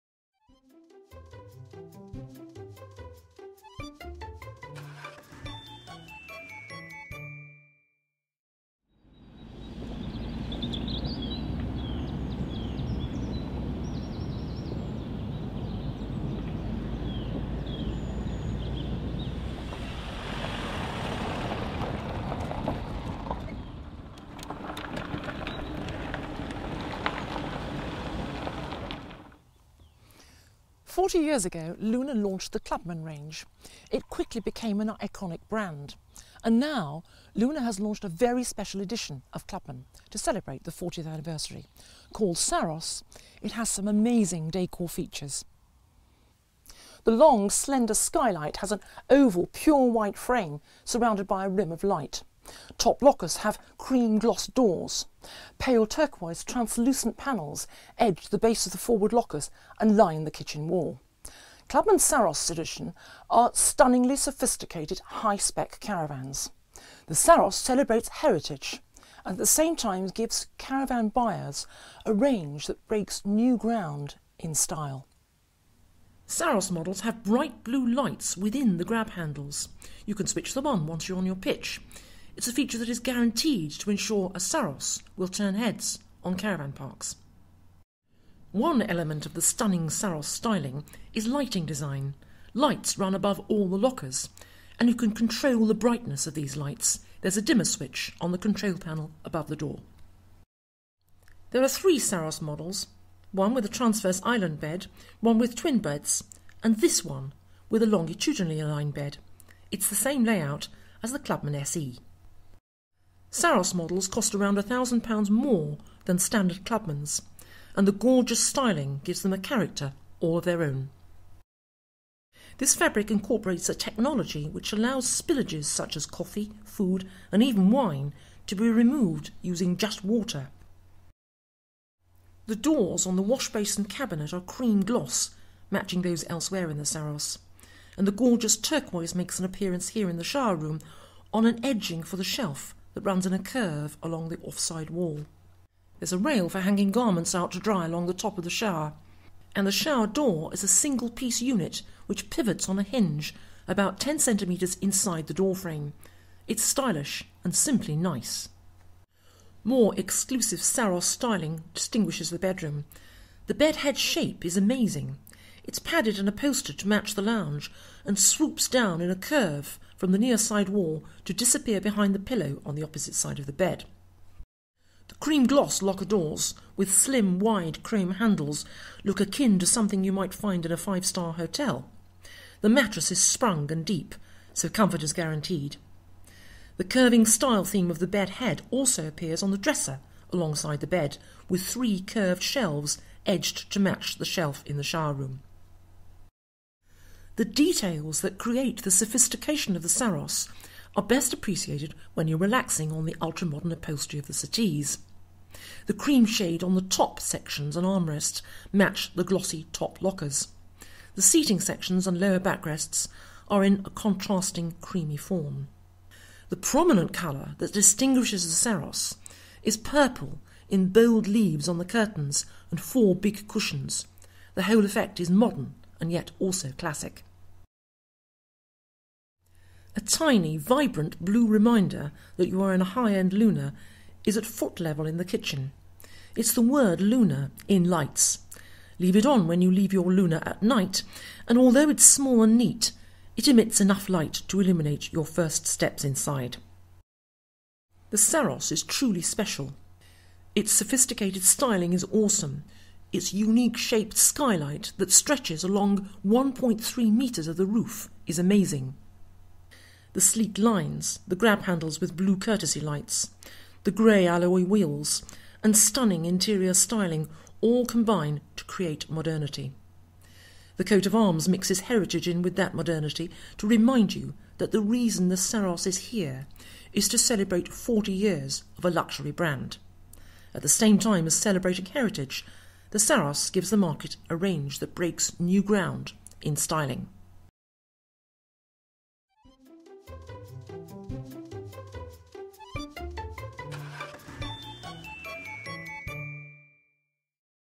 Lunar Clubman Saros SE - caravan review
Lunar celebrates 40 years of Clubman with a limited edition version of the iconic brand, called the Saros.

At the same time as celebrating heritage, the Saros gives the market a range that breaks new ground in styling.

The Lunar Clubman Saros is truly special. Its sophisticated styling is awesome. Its unique-shaped skylight that stretches along 1.3m of the roof is amazing. The sleek lines, grab handles with blue courtesy lights and grey alloy wheels that look more automotive than caravan, and stunning interior styling all combine to create modernity. The coat of arms mixes heritage in with that modernity to remind you that the reason the Saros is here is to celebrate 40 years of a luxury brand.